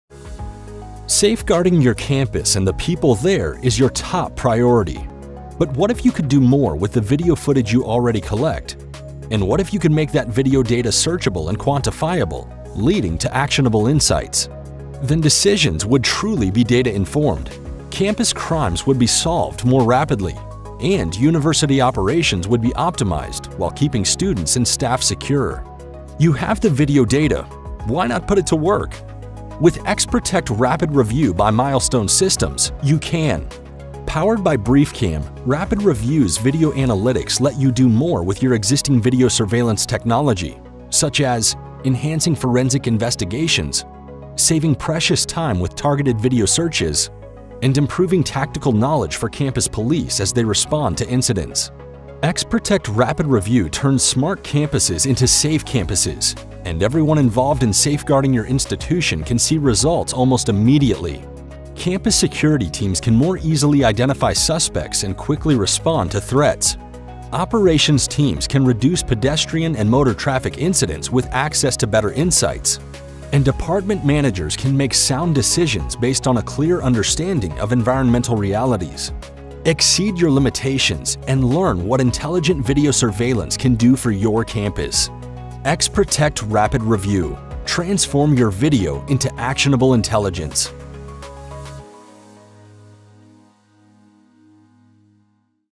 XProtect Rapid Review for Higher Education
Safeguarding your campus and the people there is your top priority. But what if you could do more with the video footage you already collect? What if you could make that video data searchable and quantifiable, leading to actionable insights? Then decisions would be truly data-informed. Campus crimes would be solved more rapidly. And, university operations would be optimized, while keeping students and staff secure.
You have the video data, why not put it to work? With XProtect® Rapid REVIEW by Milestone Systems, you can. Powered by BriefCam, Rapid REVIEW’s video analytics let you do more with your existing video surveillance technology.
Such as:
·        Enhancing forensic investigations
·        Saving precious time with targeted video searches
·        And improving tactical knowledge for campus police
         as they respond to incidents.

XProtect Rapid REVIEW turns smart campuses into safe campuses, and everyone involved in safeguarding your institution can see results almost immediately. Campus security teams can more easily identify suspects and quickly respond to threats. Operations teams can reduce pedestrian and motor traffic incidents with access to better insights. And department managers can make sound decisions based on a clear understanding of environmental realities. Exceed your limitations and learn what intelligent video surveillance can do for your campus. XProtect Rapid REVIEW –transform your video into actionable intelligence.